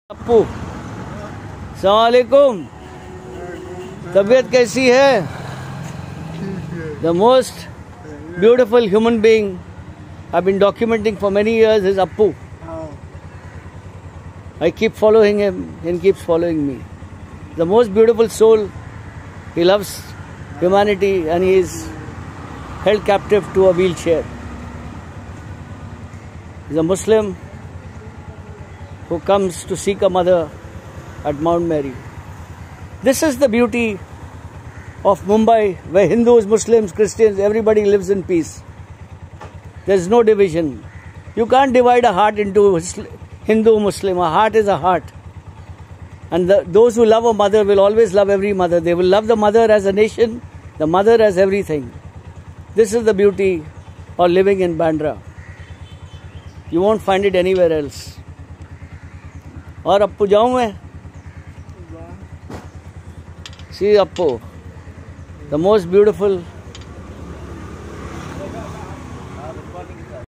Appu Muslim Beggar Invokes Mount Mary
He comes to beg every Sunday with his caretaker and the Christian folks like him he is mentally challenged to.
Probably a spastic he likes me and knows me because of my camera and also because I help him generously.